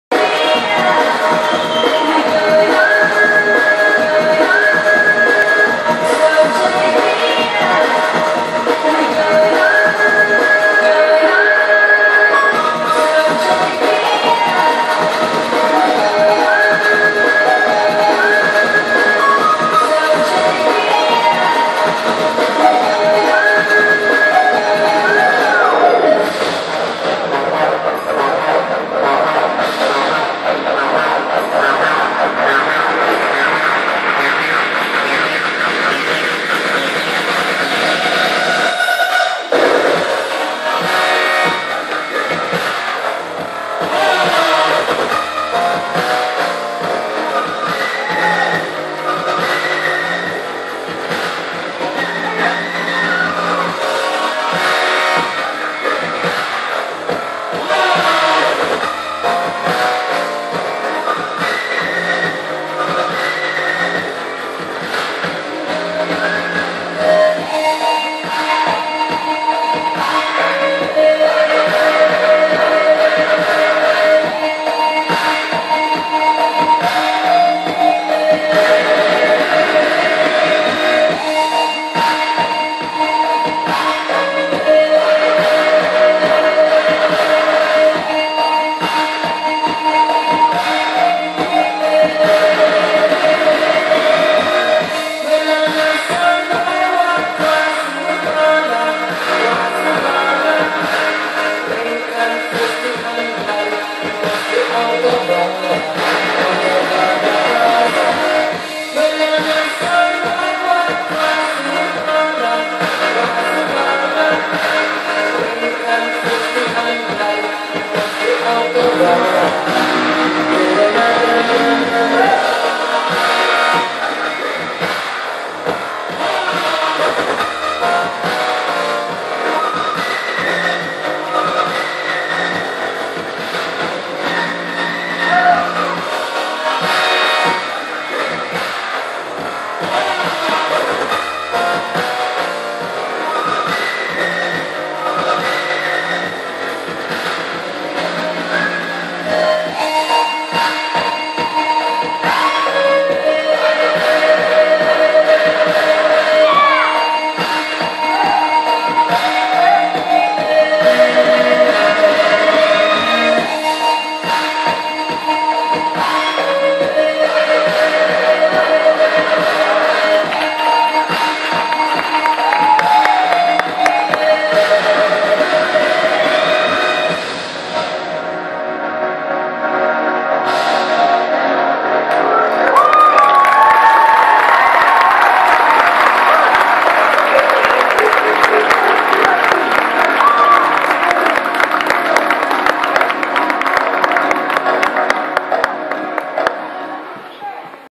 SNS demo team - 9/22/12
competing at the demo team competition in LaGrange, GA 9/22/12.